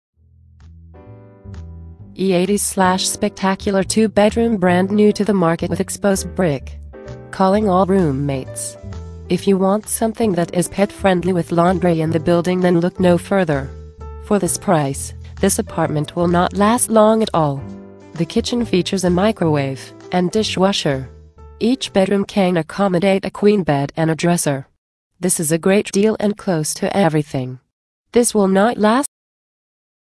Upper East Side
E. 80s/Spectacular two bedroom brand new to the market with exposed brick. Calling all roommates!If you want something that is pet friendly with laundry in the building then look no further. For this price, this apartment will not last long at all. The kitchen features a microwave, and dishwasher. Each bedroom can accomodate a queen bed and a dresser. This is a great deal and close to everything. I have ACCESS to EVERY listing FEATURED on the CitiHabitats website. I will be happy to help you find your new home.